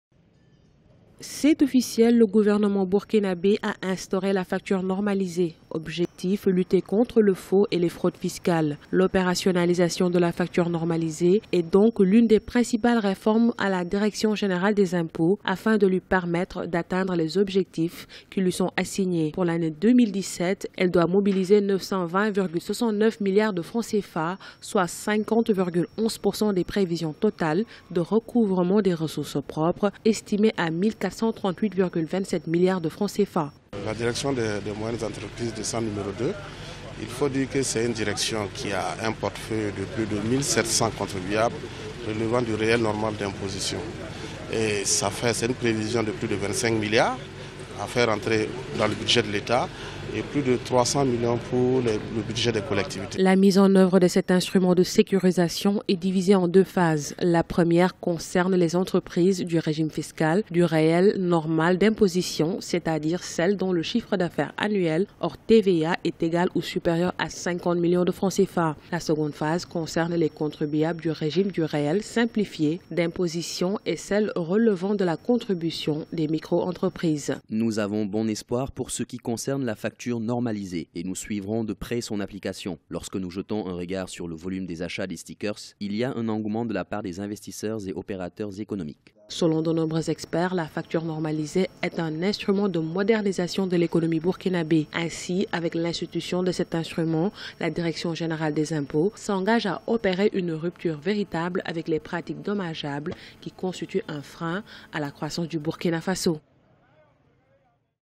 Burkina faso, LUTTE CONTRE LA FRAUDE FISCALE
Économie, l'adoption d'une facture normalisée pour booster la perception des impots et éviter les fraudes